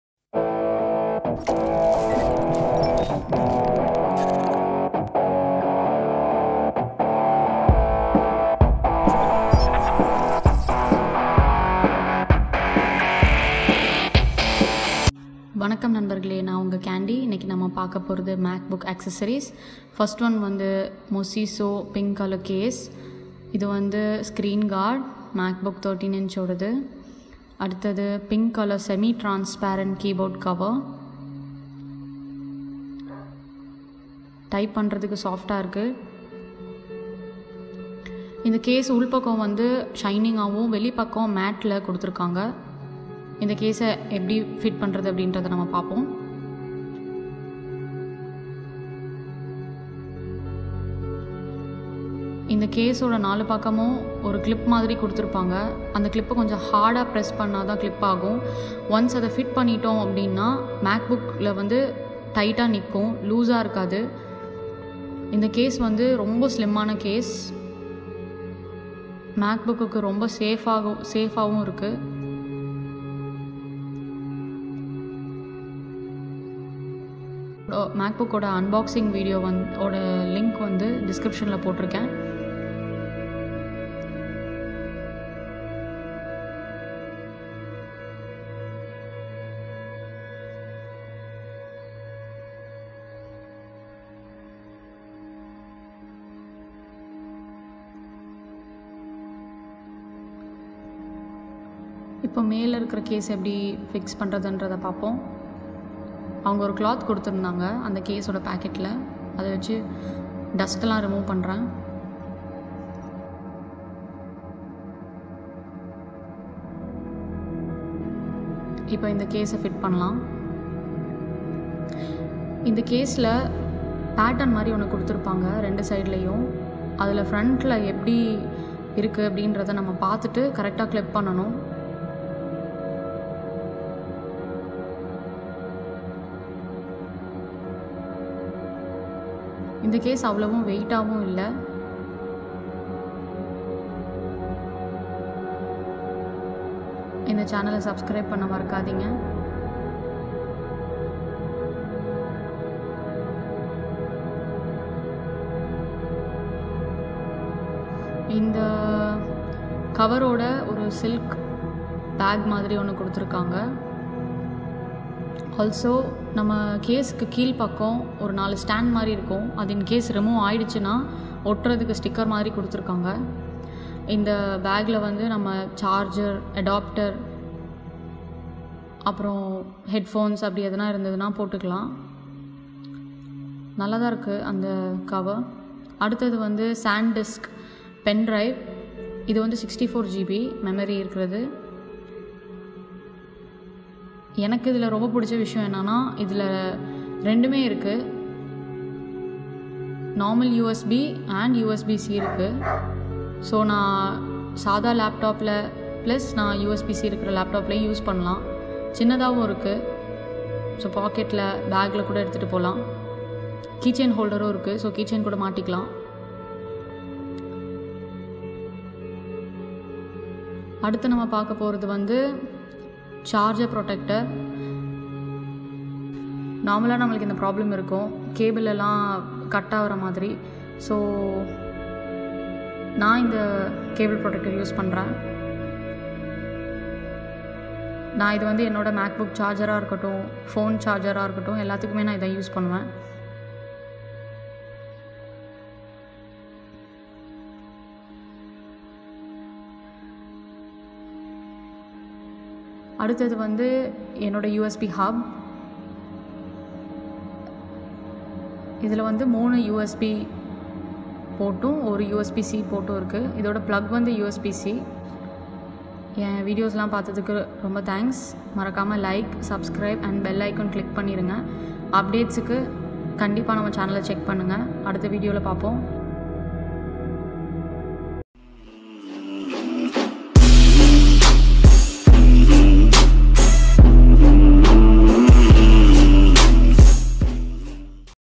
Best MacBook Pro & Air Accessories in 2020 தமிழில் | iCandy
Whats in my Laptop Bag :

These Top 10 Best Macbook Accessories lets you use your macbook to its full potential. As macbooks get  compact and slimmer each year they become more limited to accessibilities and these macbook accessories will definitely enhance your total macbook user experience.

MOSISO CASE:
SANDISK PENDRIVE:
CABLE PROTECTOR:
USB HUB: